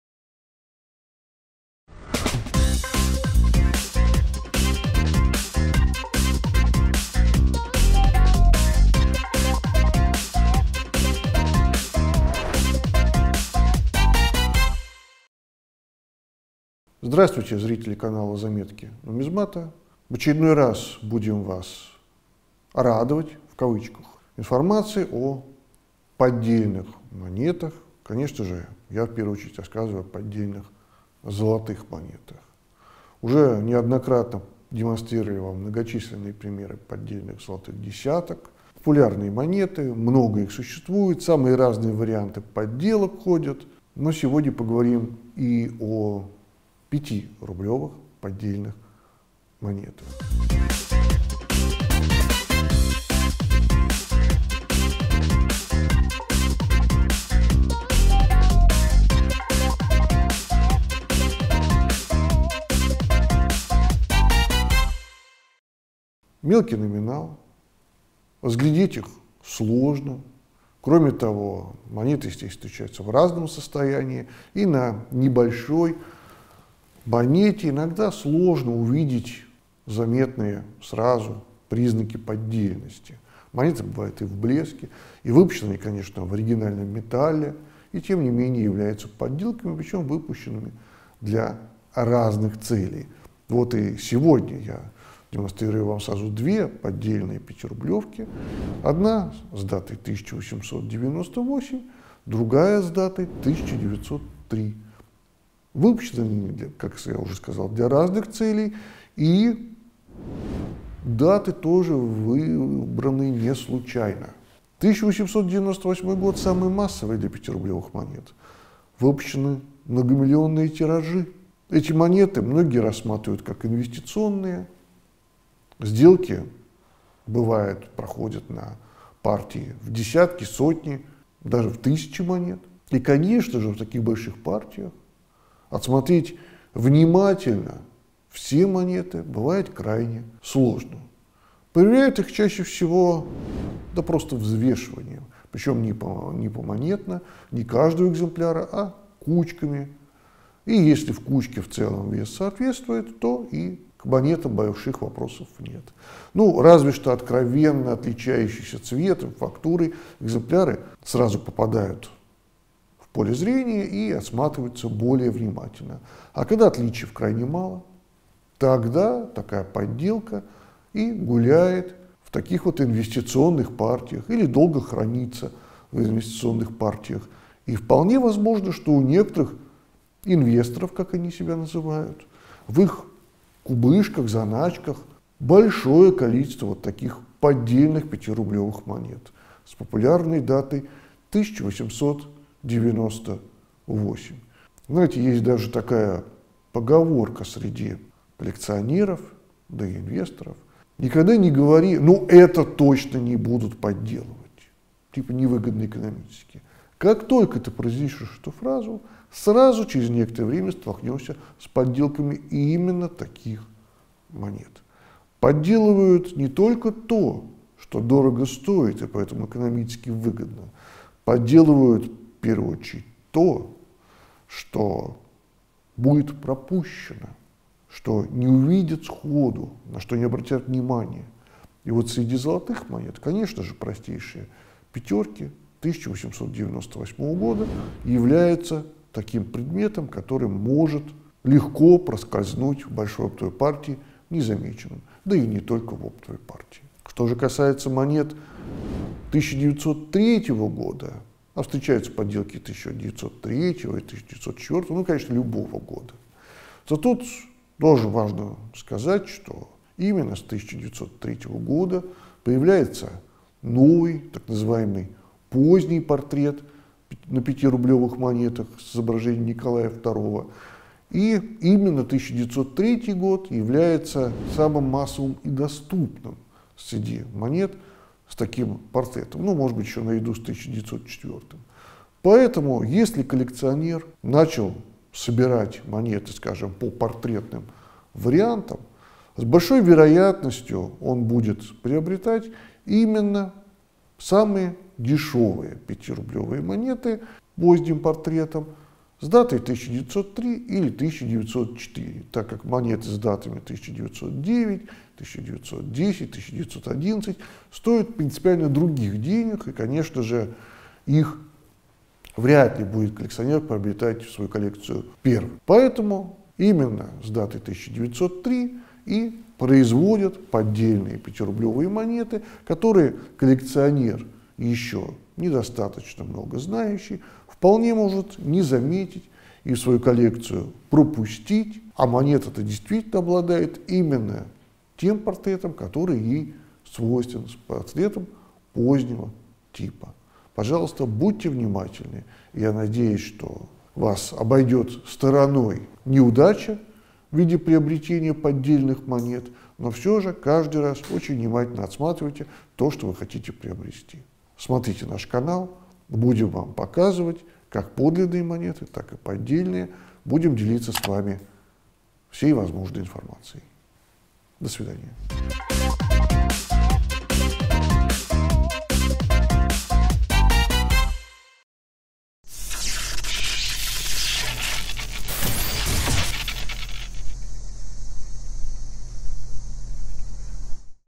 ✦ ПОДДЕЛЬНЫЕ МОНЕТЫ ПЯТИРУБЛЕВОГО ДОСТОИНСТВА ПРАВЛЕНИЯ НИКОЛАЯ ВТОРОГО ✦
Очередной рассказ Дмитрия Юрьевича Савина, посвященный золотым монетам царской России.